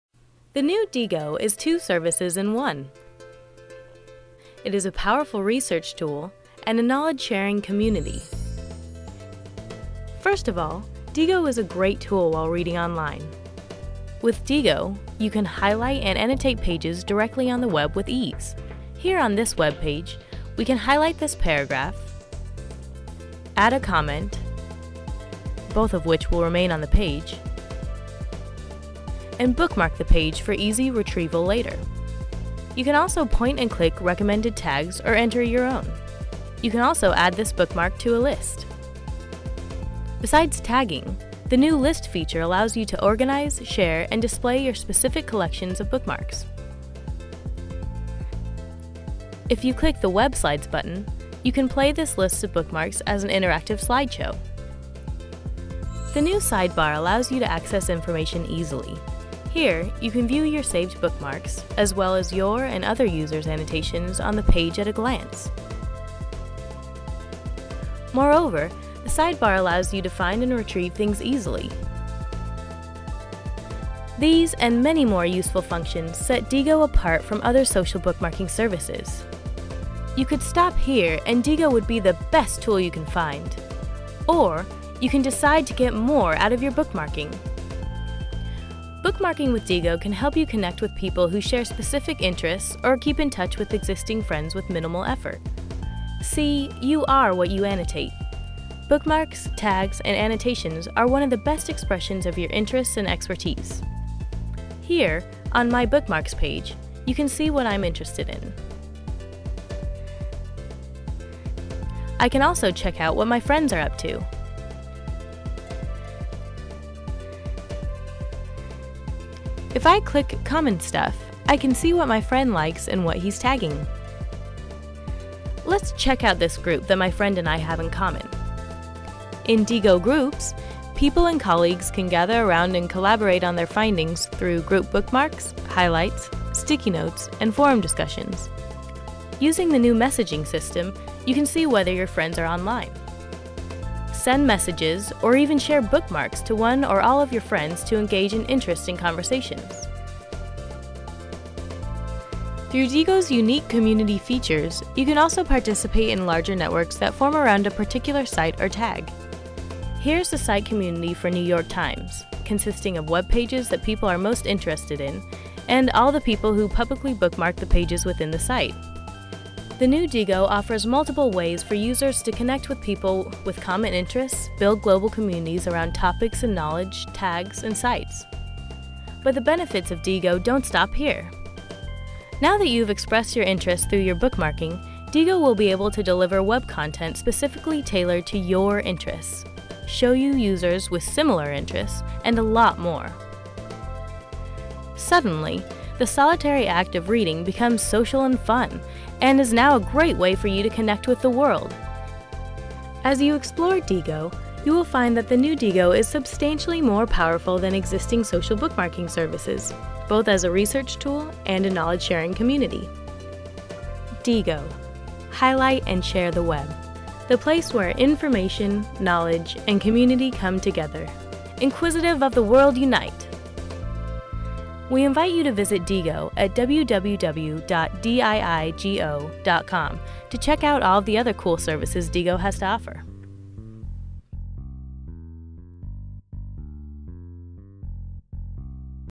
Diigo V3: Highlight & Share the Web! Social Bookmarking 2.0
Diigo ~ more than a research tool - a knowledge sharing community. Easy-to-use and so powerful... Check it out!